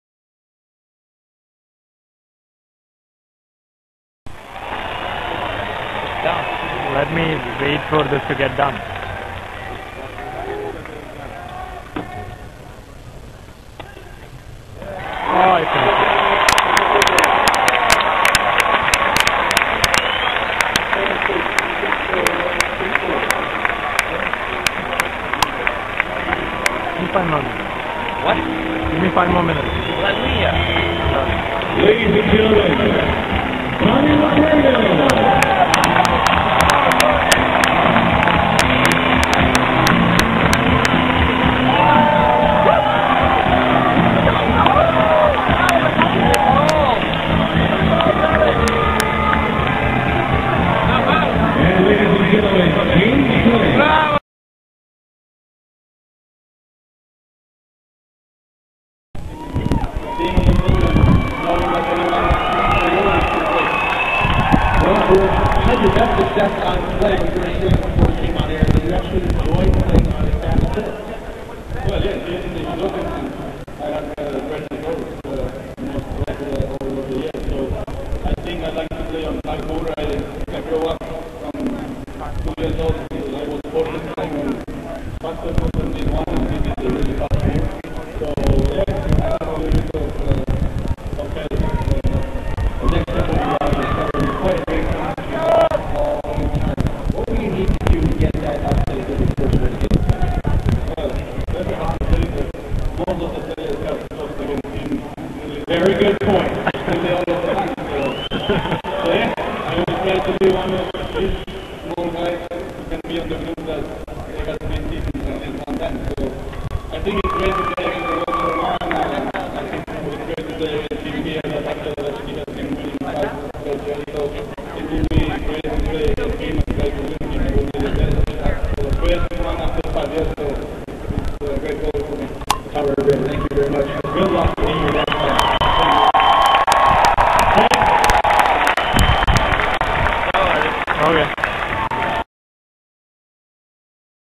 US open 2009 Robredo winning against Blake
Robredo's winning point and his thoughts on the match; men's 3rd round round match between Tommy Robredo and James Blakes